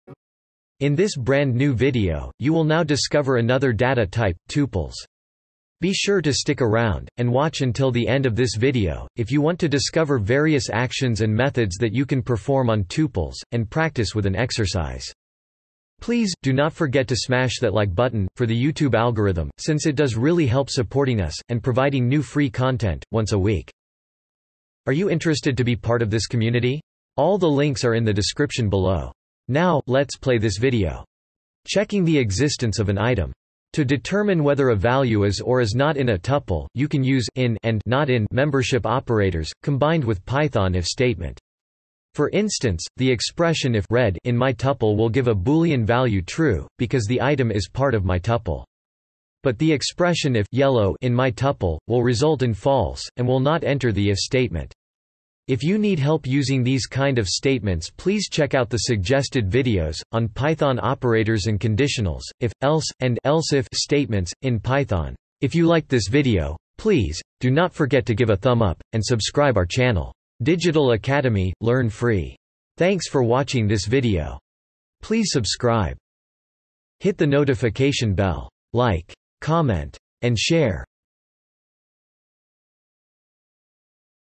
Check if an Item Exists in a Tuple in Python (IN, NOT IN) - Python Tutorial for Beginners
🖥️ Check if an Item Exists in a Tuple in Python (IN, NOT IN)

To determine whether a value is or is not in a tuple in Python, you can use "in" and "not in" membership operators, combined with Python if statement. If you need help using these kind of statements, please check out the suggested videos on Python operators and Conditionals statements in Python.

    my_tuple = ('red', 'green', 'blue')

○ Check if Item in Tuple

    if 'red' in my_tuple:
        print('red in my_tuple')

○ Check if Item NOT in Tuple

    if 'yellow' not in my_tuple:
         print('yellow not in my_tuple')

- Digital Academy™ 🎓